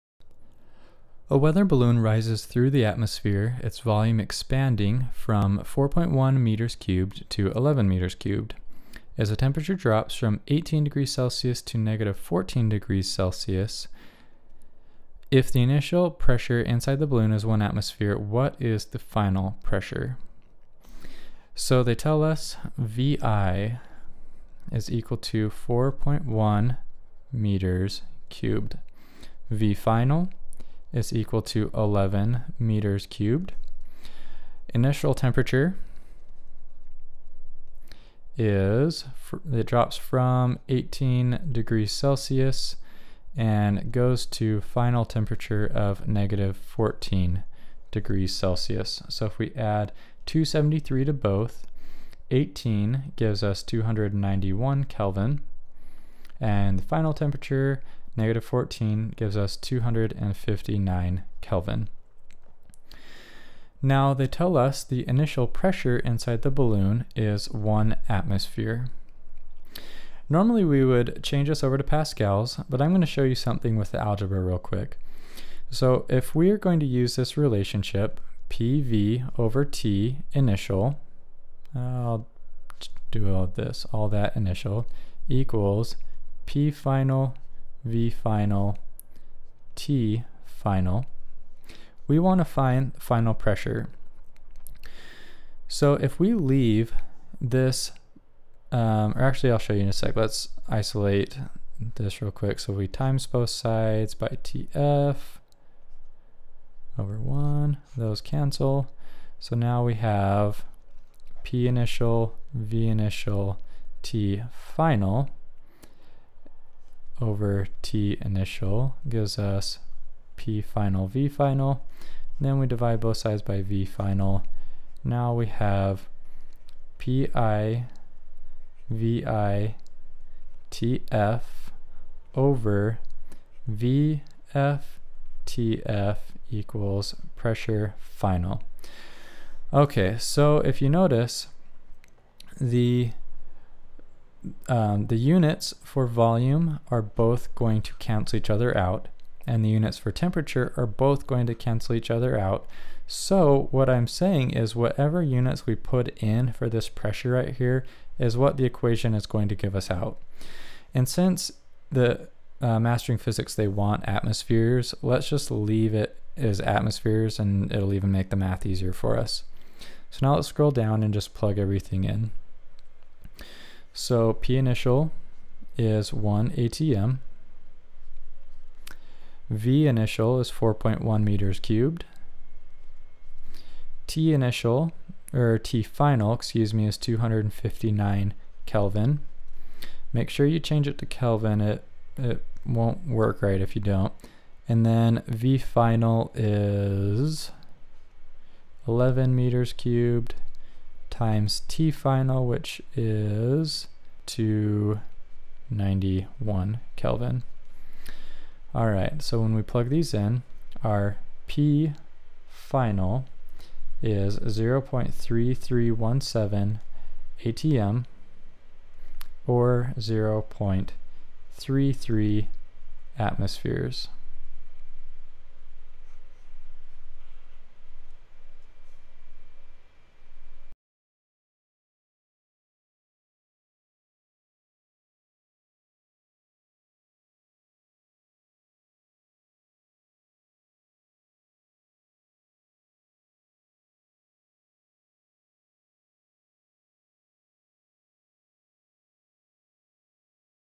Mastering Physics Solution, "A weather balloon rises through the atmosphere, its volume expanding
Mastering Physics Video Solution, "A weather balloon rises through the atmosphere, its volume expanding from 4.1 m3 to 11 m3 as the temperature drops from 18 ∘C to -14 ∘C. If the initial gas pressure inside the balloon is 1.0 atm, what is the final pressure?"

In this problem we cover how to calculate volume change due to temperature change and use that to calculate the final pressure.